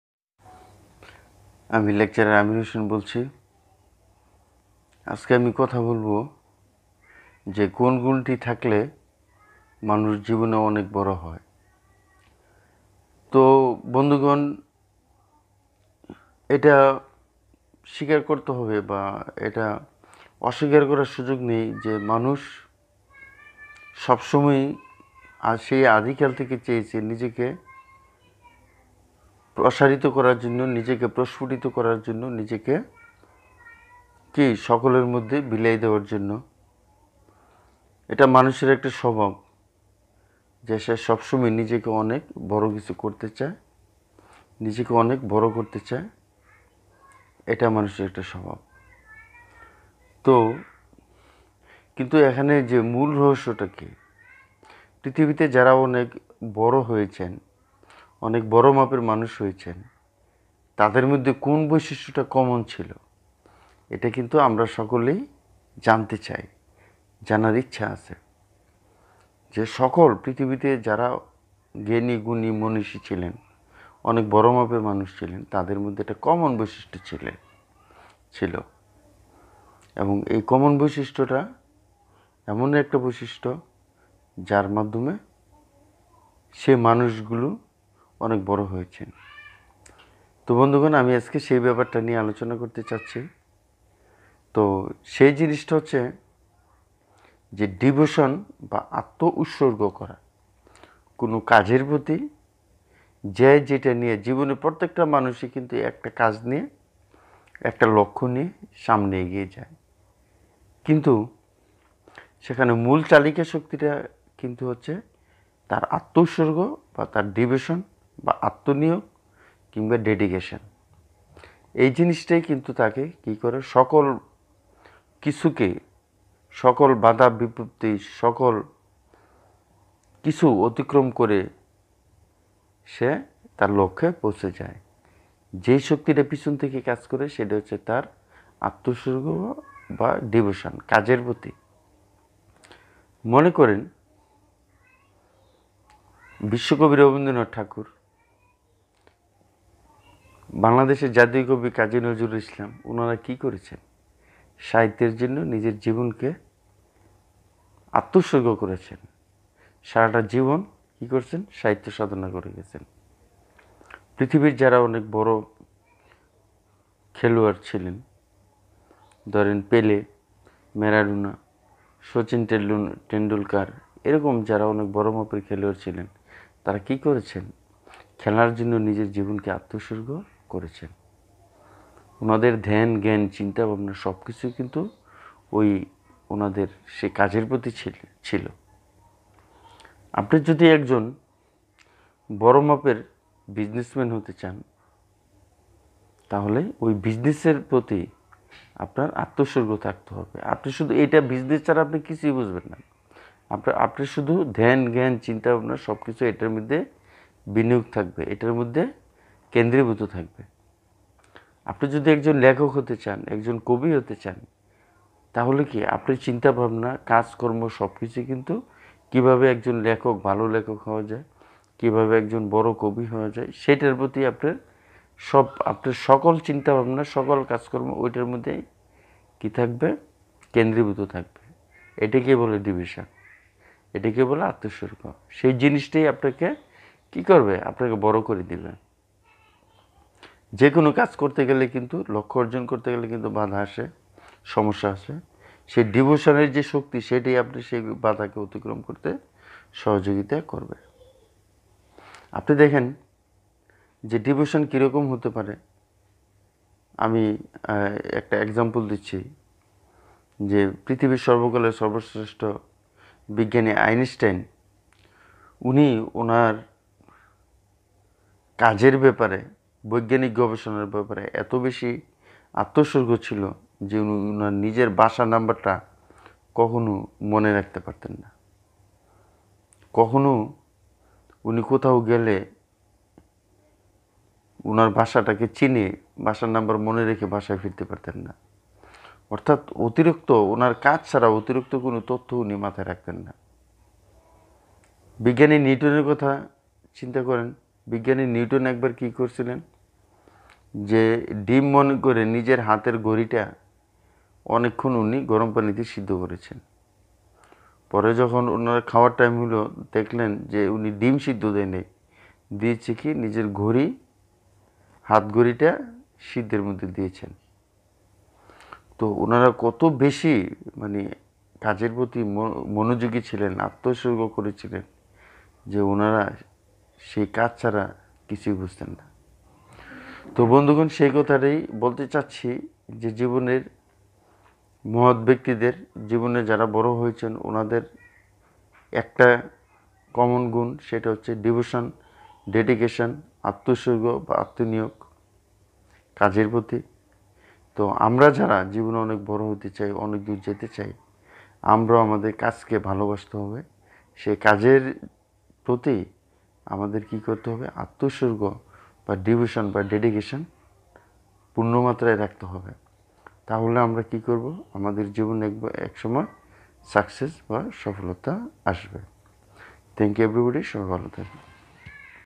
জীবনে বড় হতে হলে একটি গুন থাকতে হবে। You must have one virtue to become great in life.
Lecturer Amir Hossain describes here about a quality of great human beings.